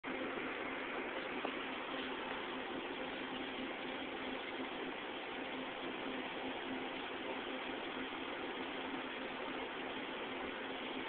Nico making a Pizza at Nico's Pizza
My son nico making a pizza at our pizzeria, Nico's Pizza